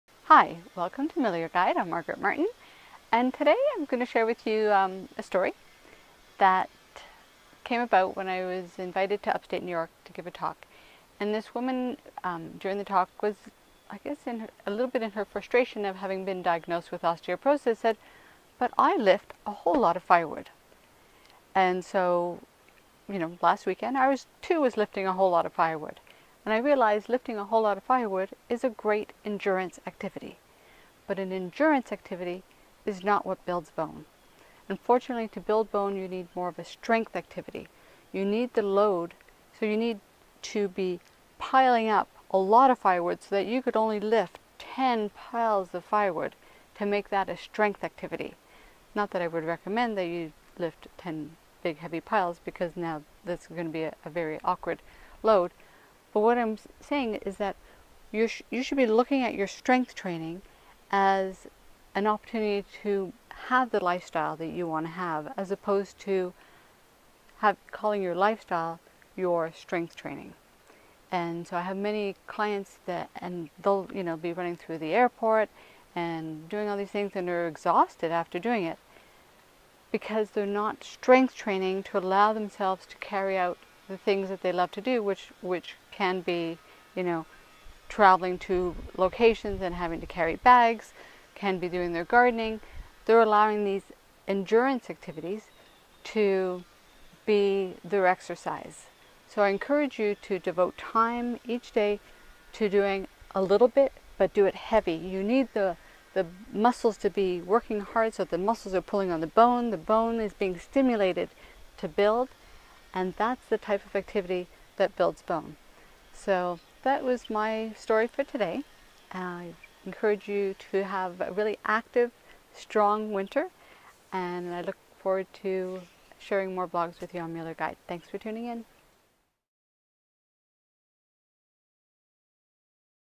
Does Improving Endurance Slow Bone Density Loss?
If you have osteoporosis and want to build bone then you should do strength training. Endurance training is good but it does not build bone.